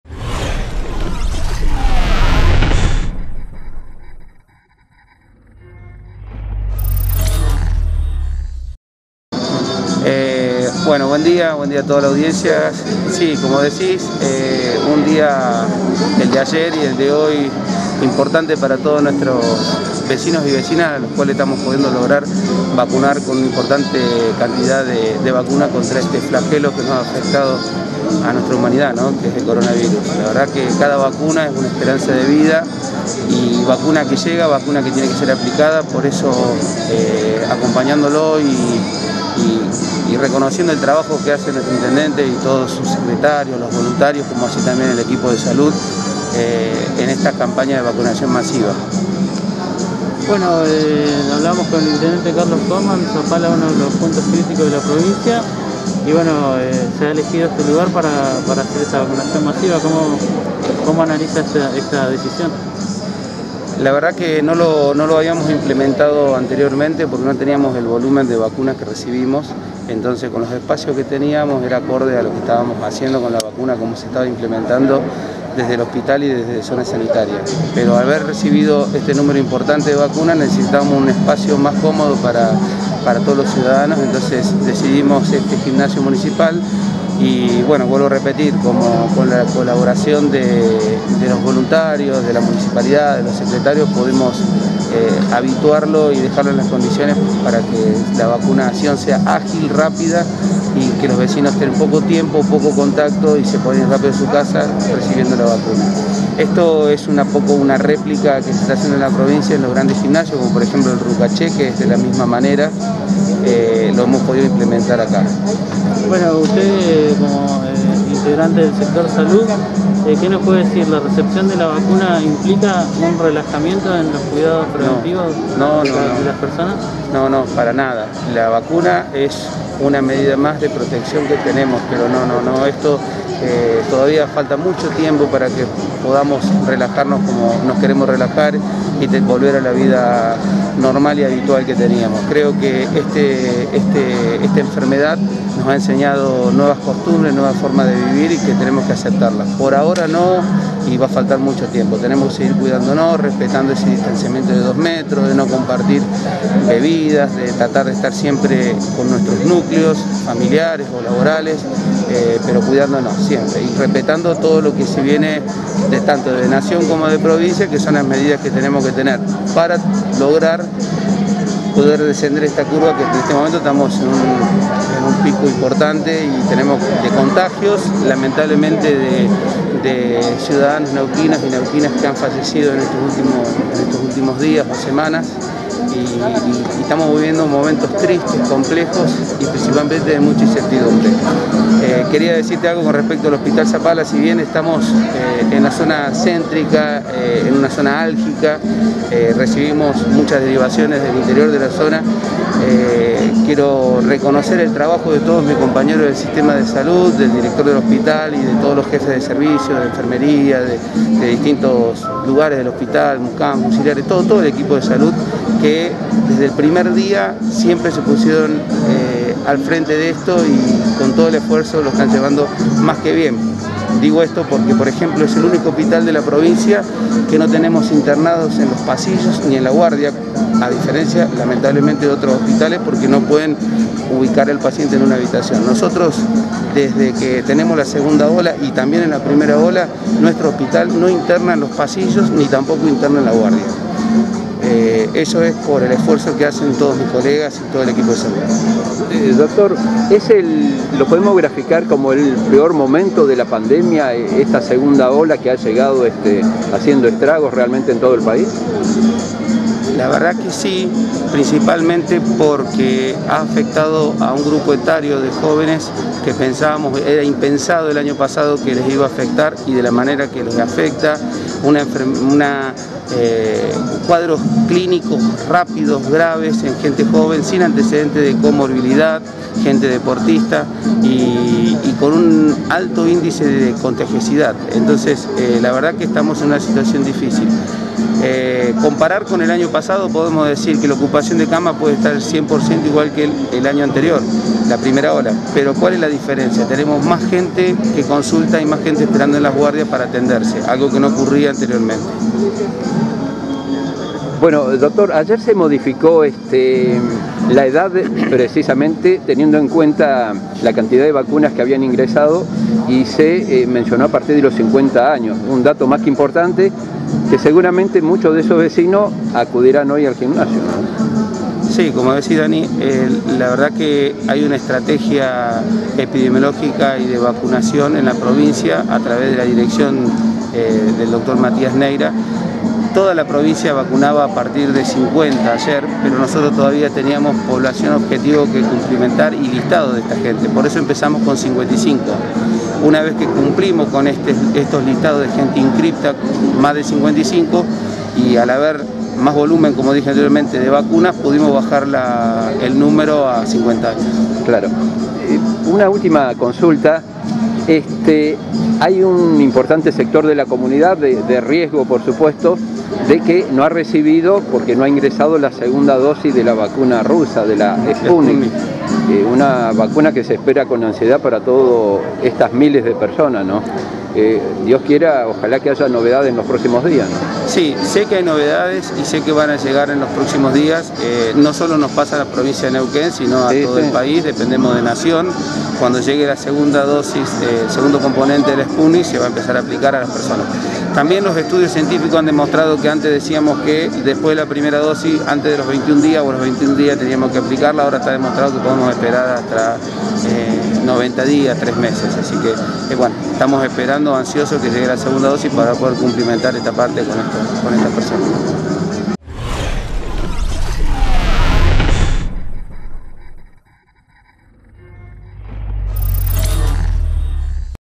DR. JAVIER RIVERO. DIPUTADO PROVINCIAL.
JORNADA DE VACUNACIÓN MASIVA EN ZAPALA.